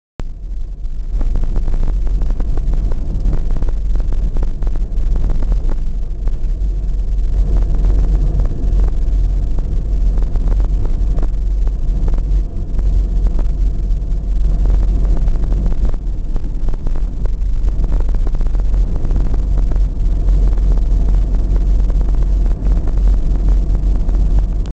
Zawiana droga zima 2012
Droga krajowa nr 11. Kiedyś był zwyczaj stawiania płotków przy drogach to takich warunków nie było.